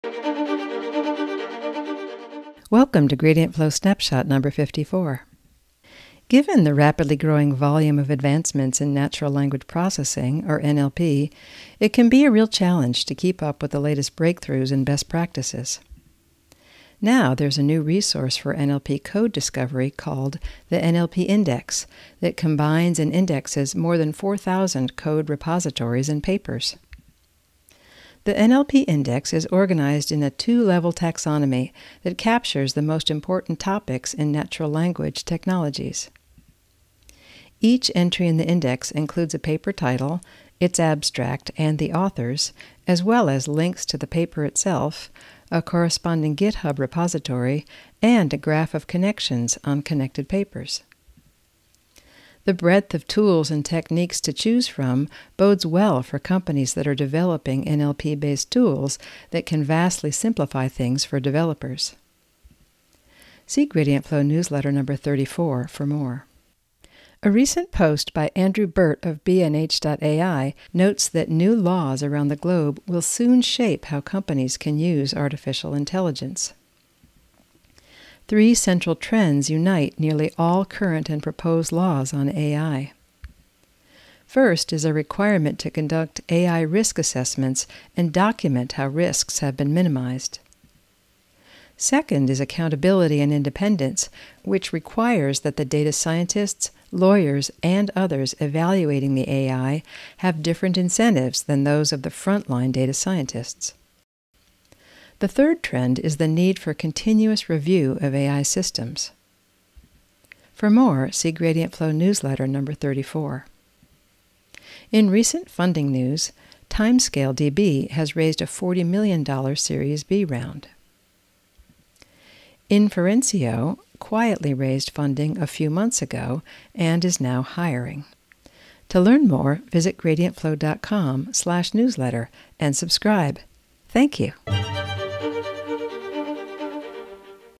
Gradient Flow Snapshot #54: NLP Index and Getting Read for New AI Regulations
- The NLP Index: a new site houses 3,000+ [code repositories + papers], organized in a 2-level taxonomy that captures the most important topics in natural language technologies.
- New AI regulations are coming … Are you ready?